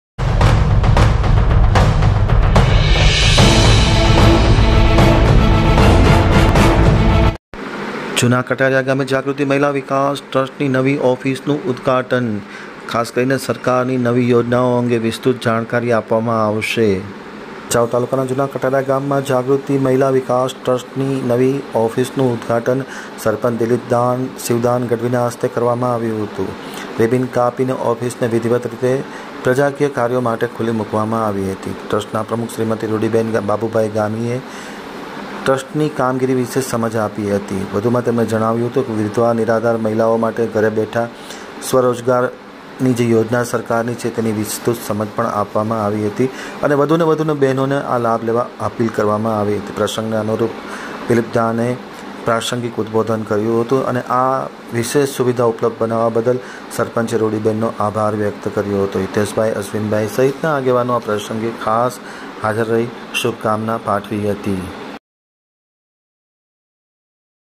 AAMTAK_NEWS જુના કટારીયા ગામે જાગૃતિ મહિલા વિકાસ ટ્રસ્ટ ની નવી ઓફીસ નું ઉદઘાટન
આજ રોજ   6/ 11 /2021 ના રોજ જુના કટારીયા ગામ મા જાગ્રુતી મહિલા વિકાસ ટ્રસ્ટ ની ઓફીસ નુ ભાઈ બીજ ના દિવસે  સરપંચ શ્રી મા,,,દિલીપદાન શિવદાન ગઢવીના હસ્તે રીબીન કાપીને ઓફિસ ના દ્વવાર  ખુલા મુકવા માં આવ્યા હતા આ દરમીયાન ટ્રસ્ટ ના પ્રમુખ શ્રીમતી  રૂડીબેન બાબુભાઈ ગામીએ ટ્સ્ટની કામગીરી અંગે સમજ આપી હતી વધુ મા રૂડીબેને  જણાવ્યુ  હતુ  વિધવા નિર્આધાર  મહિલાઓ માટે ઘરે બેઠા સ્વ રોજગાર  મડી રહે એ અંગે સરકાર શ્રીની યોજનાઓ અંગે સમજ આપી હતી આ પ્રસંગ  સરપંચ શ્રી દિલીપદાન અને સંસ્થા  ના પ્રમુખ રૂડીબેન  ટ્રસ્ટીઓ અને ગામ જનો મીઠીબેન હિતેસ ભાાઈ અશ્વિન ભાઇ વિગેરે મોટી સંખ્યામા ભાઈઓ તથા બેનો હાજર રહ્યા હતા